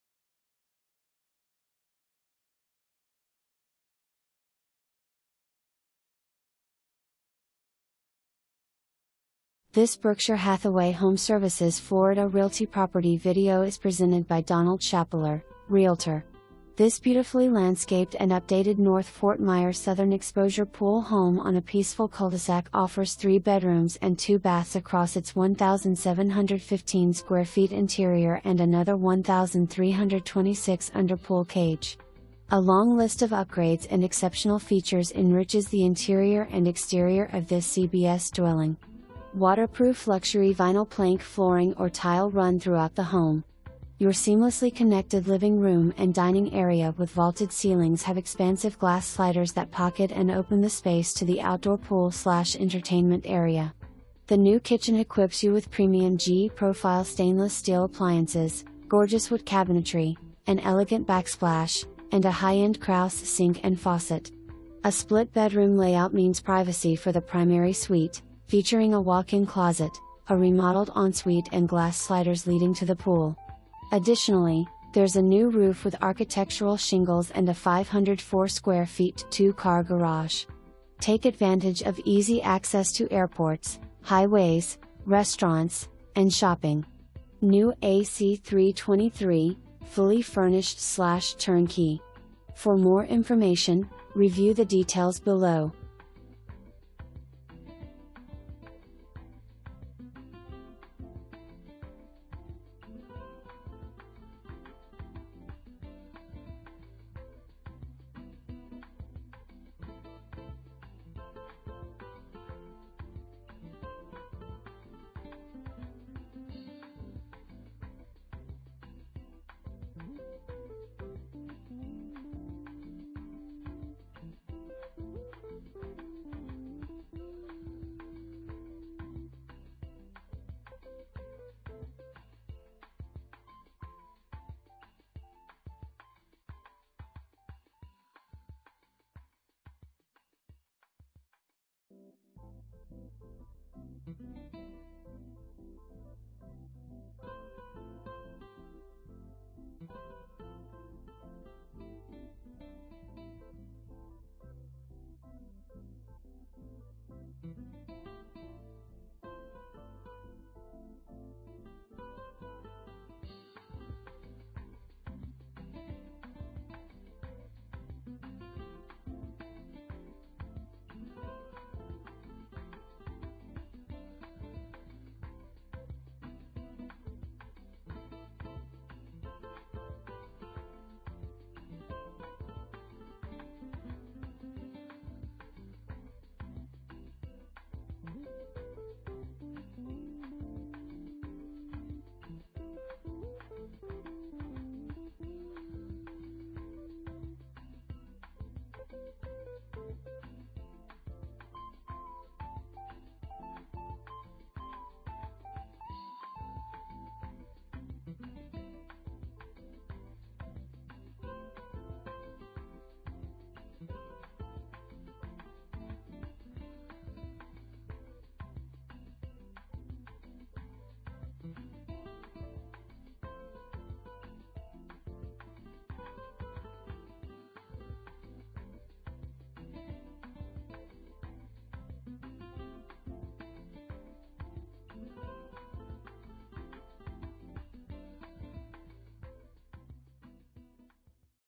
Berkshire Hathaway HomeServices Florida Realty - 15340 Sam Snead Lane
This beautifully landscaped and updated North Fort Myers Southern exposure pool home on a peaceful cul-de-sac offers 3 bedrooms and 2 baths across its 1,715-sq ft interior and another 1326 under pool cage. A long list of upgrades and exceptional features enriches the interior and exterior of this CBS dwelling. Waterproof luxury vinyl plank flooring or tile run throughout the home. Your seamlessly connected living room and dining area with vaulted ceilings have expansive glass sliders that pocket and open the space to the outdoor pool/ entertainment area. The new kitchen equips you with premium GE Profile stainless steel appliances, gorgeous wood cabinetry, an elegant backsplash, and a high-end Kraus sink and faucet. A split-bedroom layout means privacy for the primary suite, featuring a walk-in closet, a remodeled ensuite and glass sliders leading to the pool. Additionally, there’s a new roof with architectural shingles and a 504-sq ft 2-car garage. Take advantage of easy access to airports, highways, restaurants, and shopping. [ NEW A/C 3/23 ] Fully furnished/turnkey.

2 full bath

Donald Chappelear, Realtor®
Berkshire Hathaway HomeServices Florida Realty